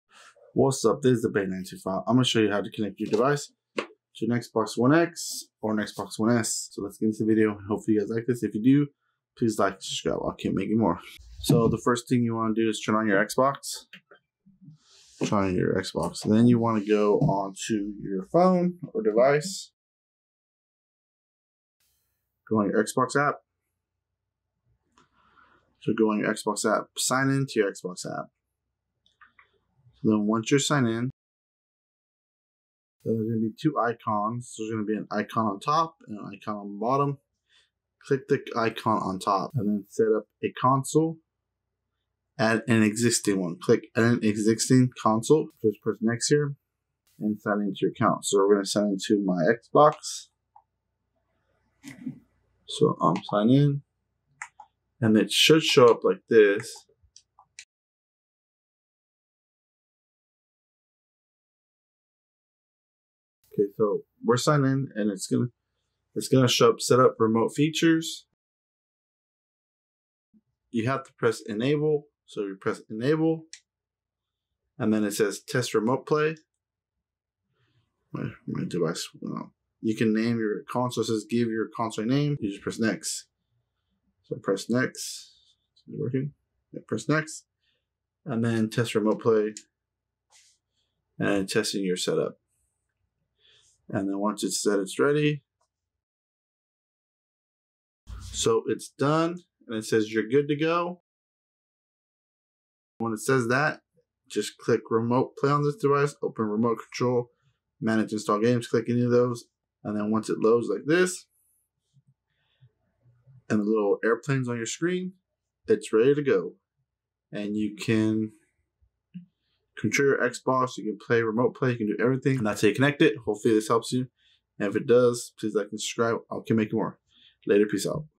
How to connect your smartphone to your Xbox One,X,S with the Xbox app updated video 2020
This video is an updated video on how to connect a smart device to you Xbox one,x,s. In this video I connect my smartphone to my Xbox one x. Enjoy